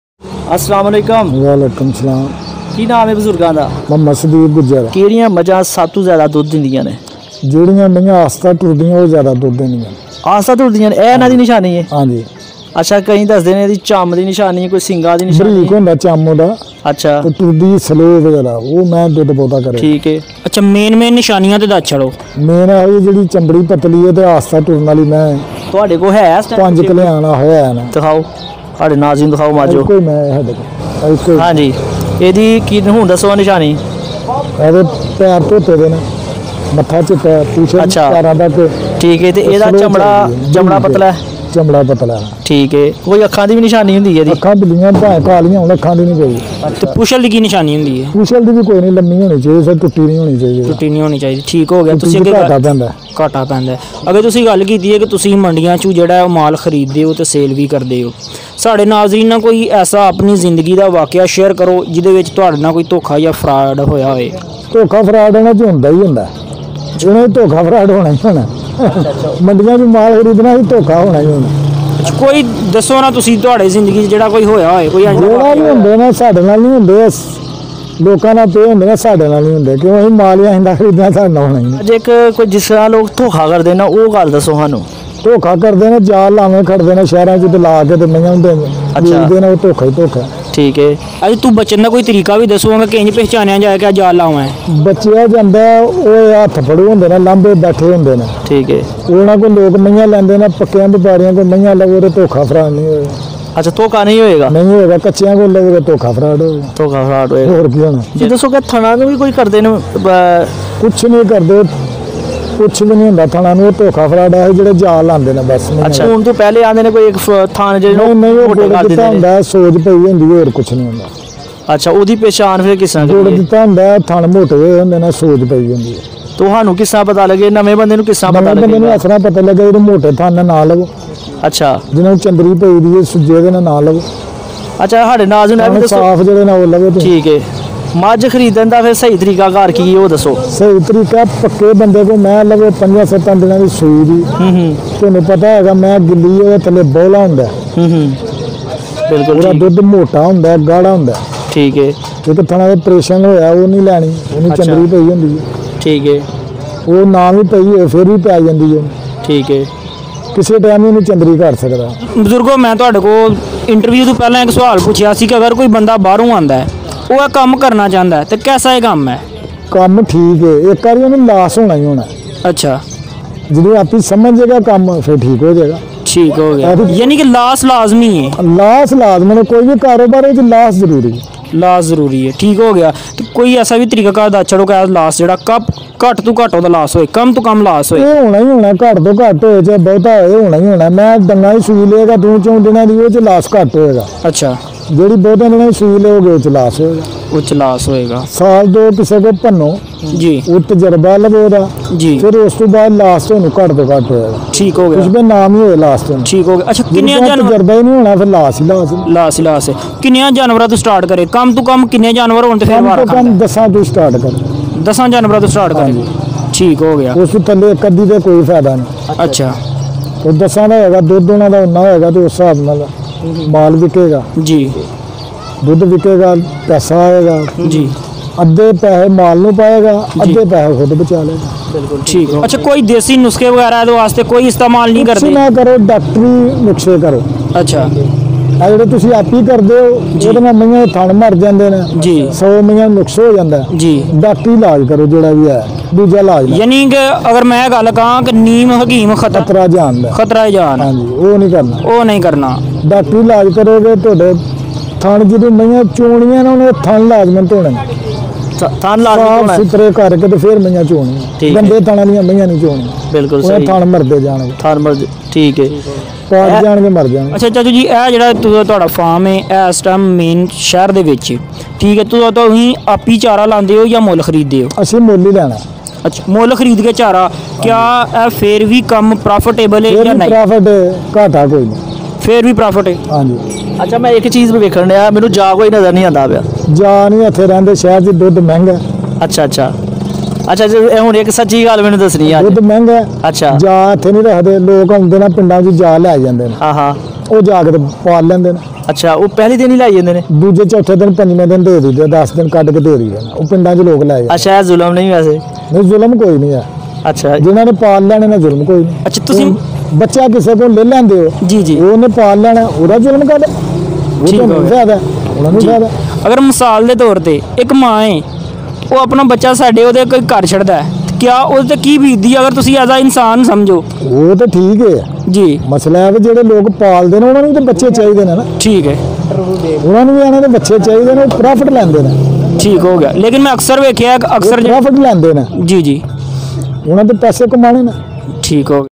Buffalo Farm In Pakistan||Buffalo Dairy Farm In Pakistan|Buffalo Farming In Pakistan
In this video you can see how to buy buffalo in buffalo mandi. How to runn buffalo farm in pakistan.